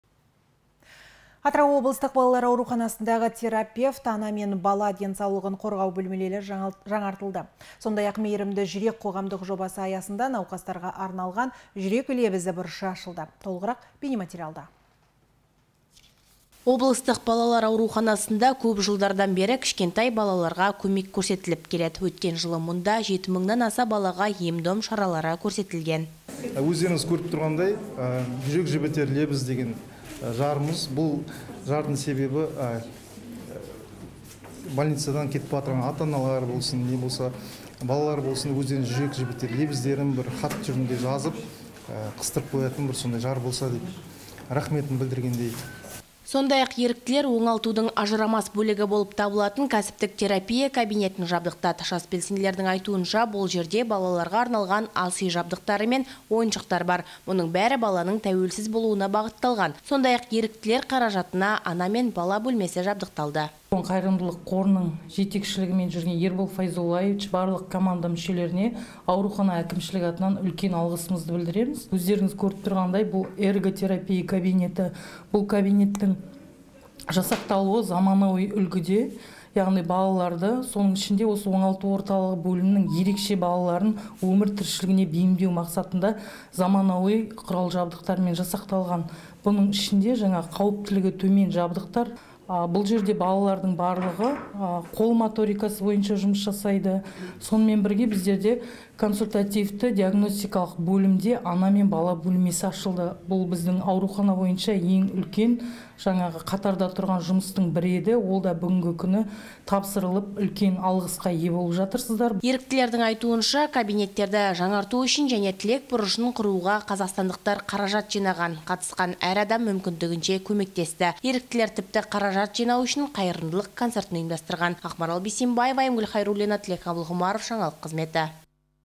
БАЛАЛАРҒА АРНАЛҒАН «ЖҮРЕК ЛЕБІЗІ» БҰРЫШЫ АШЫЛДЫ
Атырау облыстық балалар ауруханасындағы терапевт, ана мен бала денсаулығын қорғау бөлмелері жаңартылды. Сондай-ақ «Мейірімді жүрек» қоғамдық жобасы аясында науқастарға арналған «Жүрек лебізі» бұрышы ашылды. Толығырақ бейнематериалда.